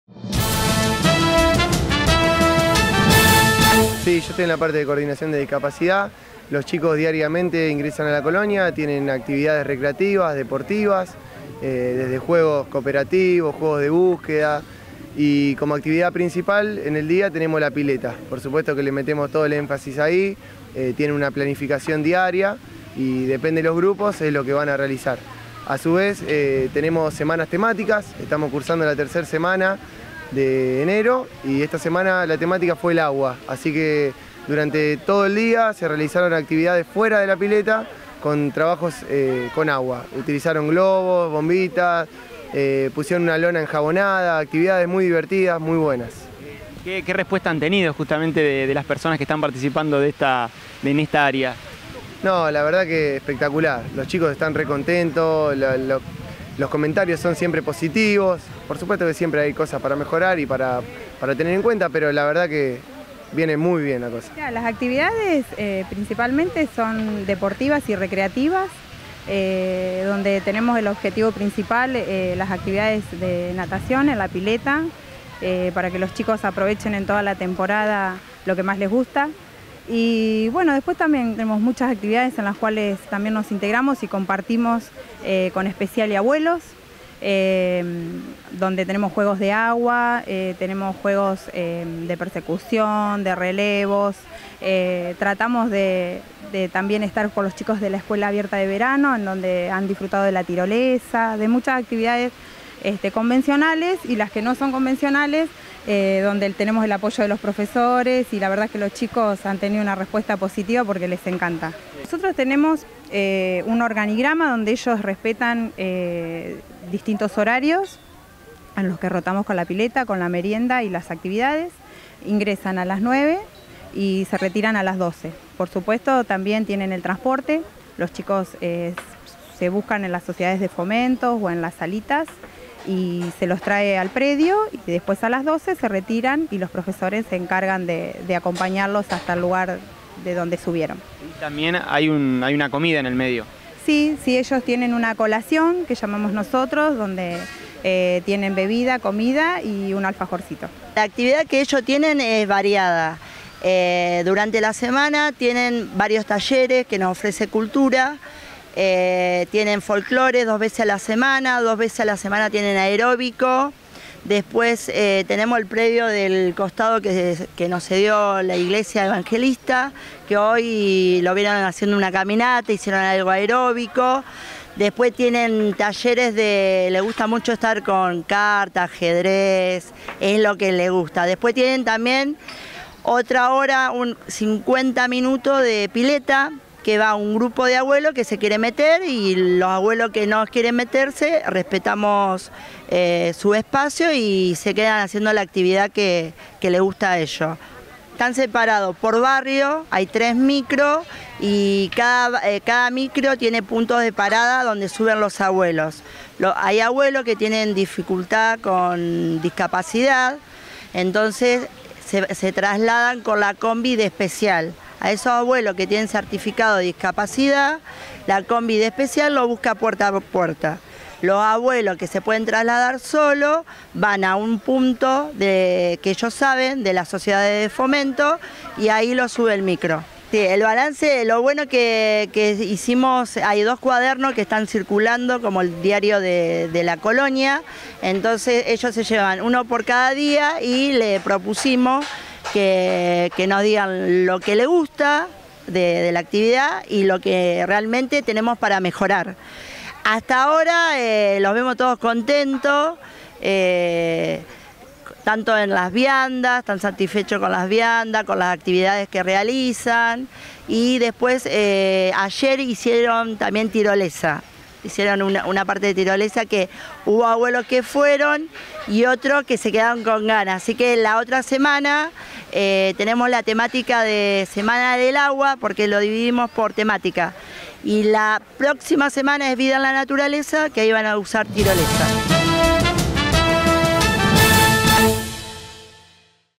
Colonia integradora
Se desarrolla en el Club Puerto Nuevo. Niños, adultos mayores y jóvenes con discapacidad comparten actividades recreativas desde el 2 de enero.